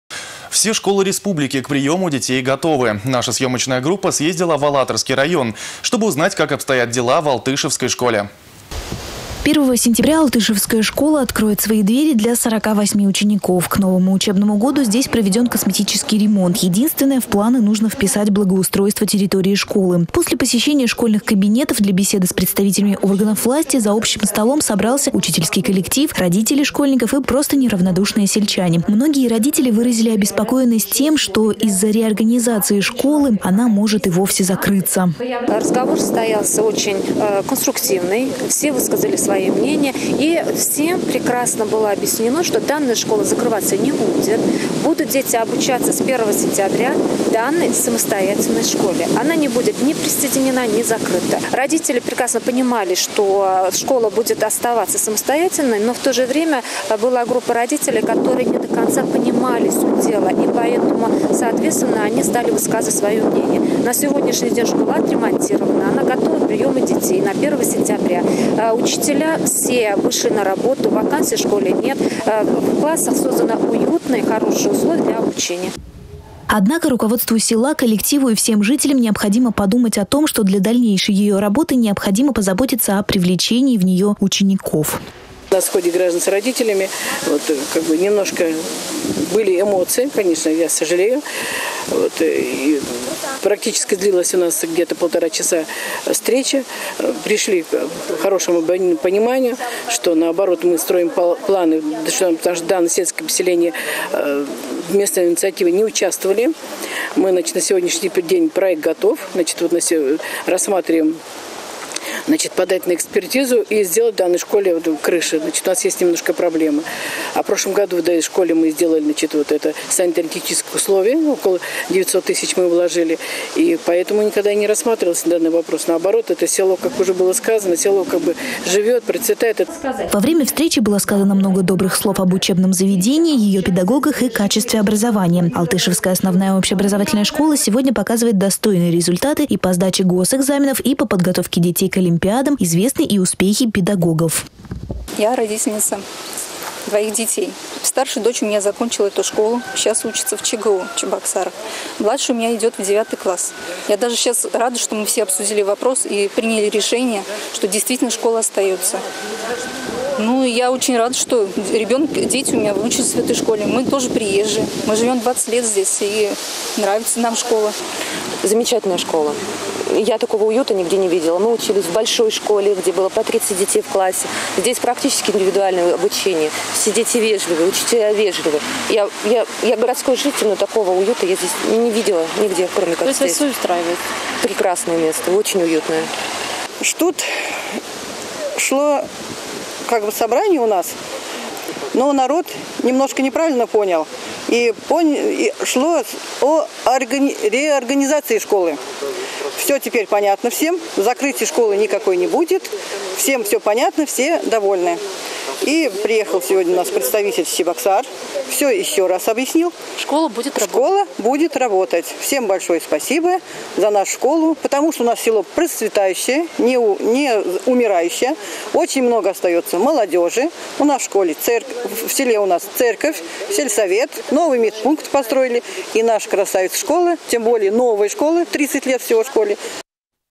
Все школы республики к приему детей готовы
Наша съемочная группа съездила в Алатырский район, чтобы узнать, как обстоят дела в Алтышевской школе.